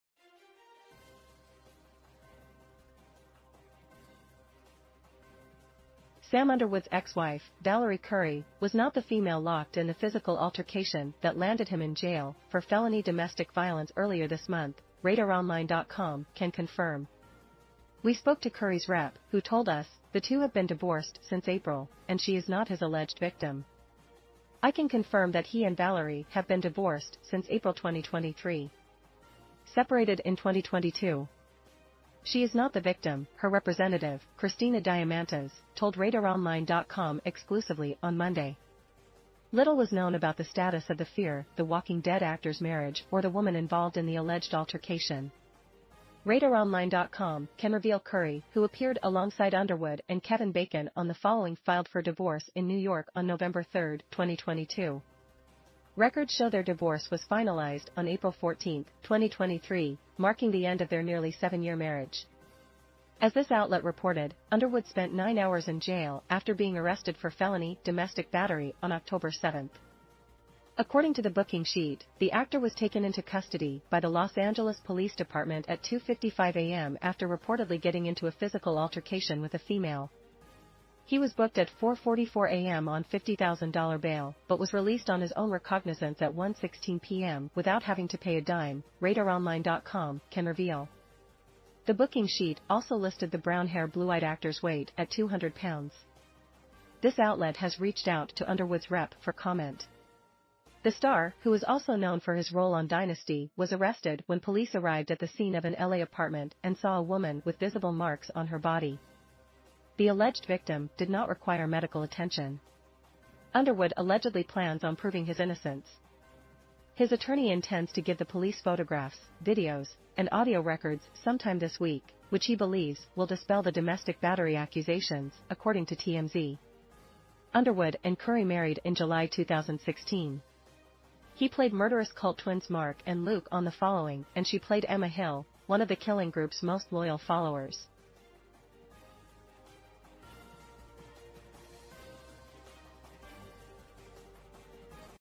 'Fear the Walking Dead' Star Sam Underwood's Ex-Wife Valorie Curry Not Alleged Victim in Felony Dome
Sam Underwood's ex-wife, Valorie Curry, was not the female locked in the physical altercation that landed him in jail for felony domestic violence earlier this month, RadarOnline.com can confirm.